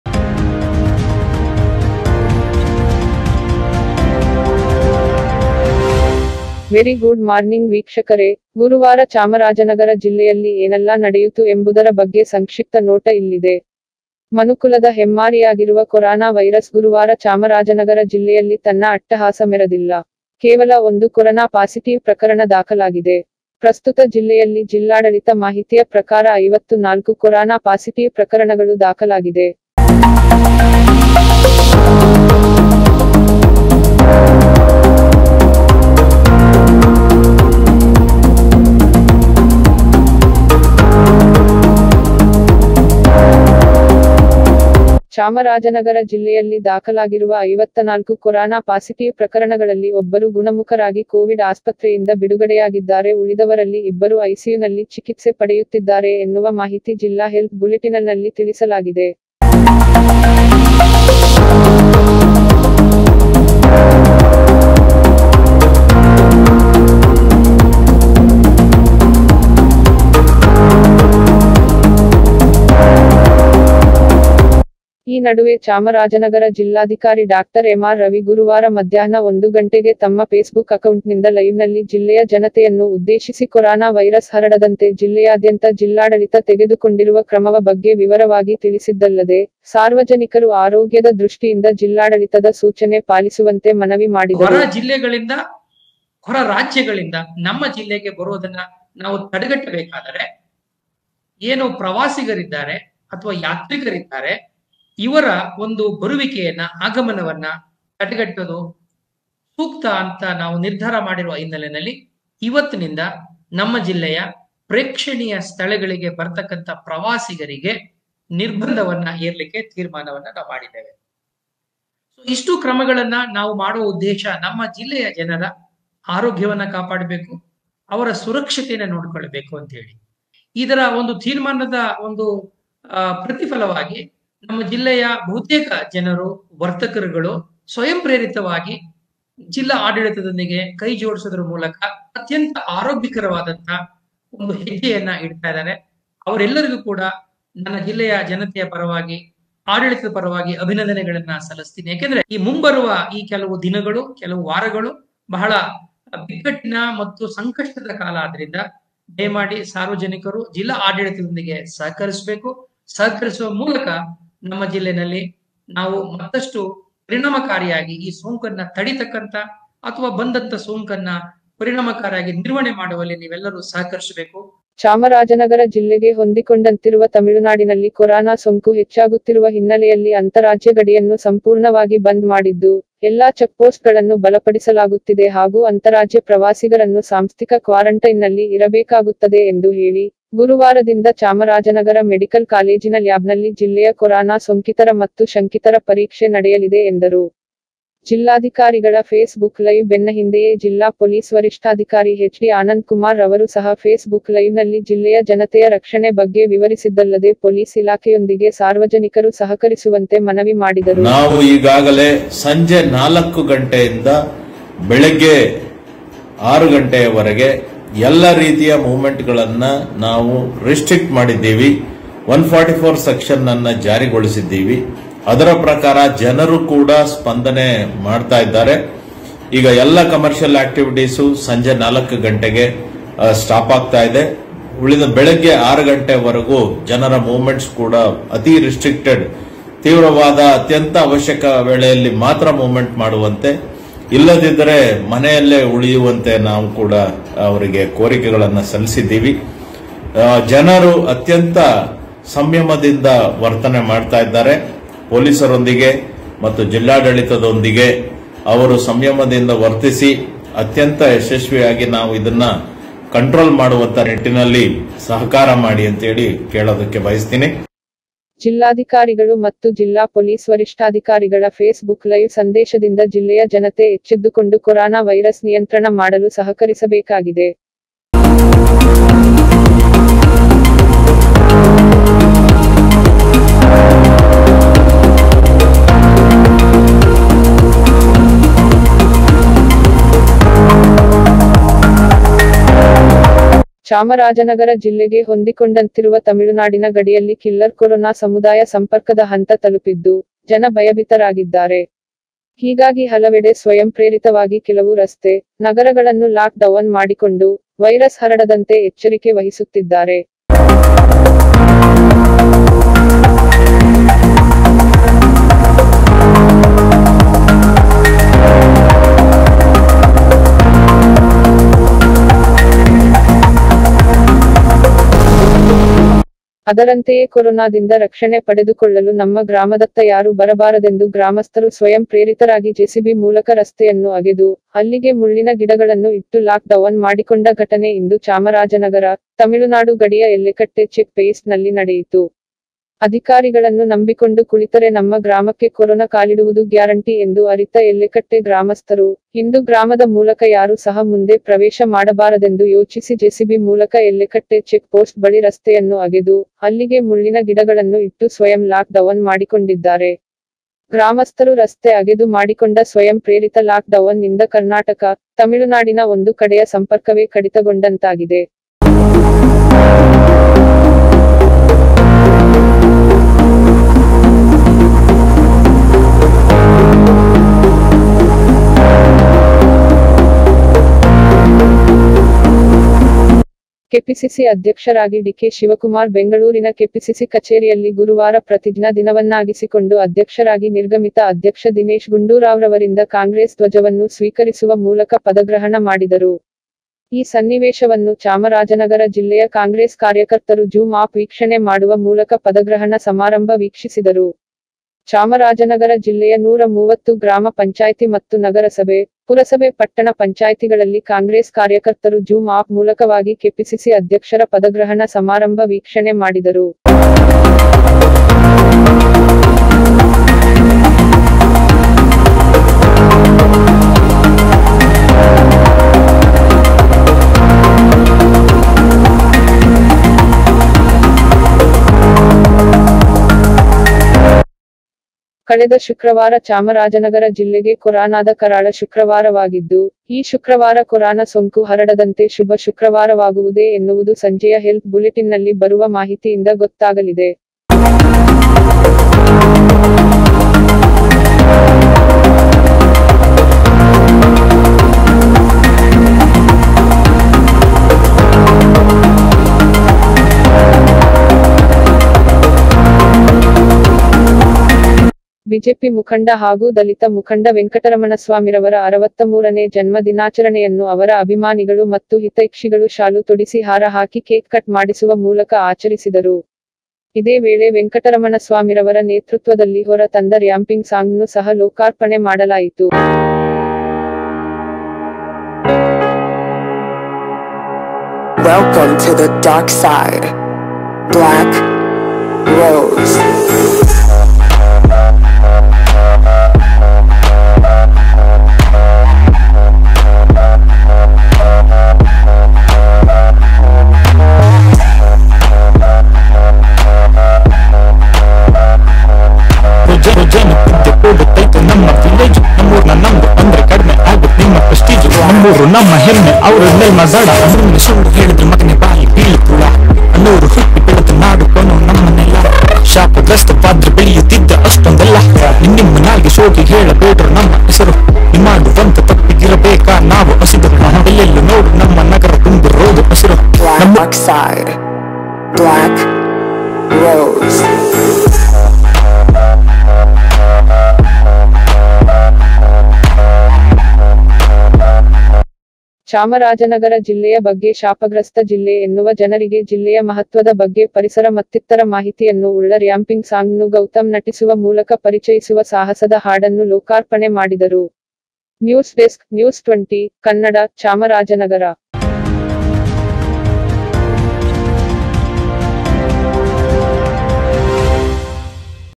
CHAMARAJANAGAR # MORNING NEWS
ಶುಭೋದಯ ವೀಕ್ಷಕರೇ.

ಚಾಮರಾಜನಗರ ಜಿಲ್ಲೆಯಲ್ಲಿ ಗುರುವಾರ ಏನೆಲ್ಲಾ ಬೆಳವಣಿಗೆ ಆಯ್ತು ಅಂತ ಸಂಕ್ಷಿಪ್ತ ವರದಿ

ಚಾಮರಾಜನಗರ ಜಿಲ್ಲೆಗೆ ಗುರುವಾರ ಶುಭ ಗುರುವಾರವಾಗಿದೆ.

ಗುರುವಾರ ಸಂಜೆ ತನಕ ಕೇವಲ ಒಂದುಕೊರಾನಾ ಸೊಂಕು ದೃಡ

ಅಂತರಾಜ್ಯ ಗಡಿಯಲ್ಲಿ ಕೊರಾನಾ ಸೊಂಕು ನಿಯಂತ್ರಣಕ್ಕೆ ಫುಲ್ ಸೆಕ್ಯೂರಿಟಿ

ಅಂತರಾಜ್ಯ ಪ್ರವಾಸಕ್ಕೆ ಬರುವ ಪ್ರವಾಸಿಗರಿಗೂ ಕ್ವಾರಂಟೈನ್

ಜಿಲ್ಲಾಧಿಕಾರಿ ಮತ್ತು ಪೊಲೀಸ್ ವರಿಷ್ಠಾಧಿಕಾರಿಗಳ ಫೇಸ್ ಬುಕ್ ಲೈವ್

ತಾಳವಾಡಿ ಚಿಕ್ಕಹೊಳೆ ನಡುವಿನ ಎಲ್ಲೆಕಟ್ಟೆಯಲ್ಲಿ ಕೊರಾನಾ ಭೀತಿಯಿಂದ ರಸ್ತೆ ಅಗೆದು ಸಂಚಾರ ಬಂದ್
ದಲಿತ ಮುಖಂಡರ ಜನ್ಮ ದಿನದಂದು ಜಿಲ್ಲೆಯ ವೈಶಿಷ್ಟತೆ ಬಗ್ಗೆ ರ್ಯಾಂಪಿಂಗ್ ಸಾಂಗ್ ಬಿಡುಗಡೆ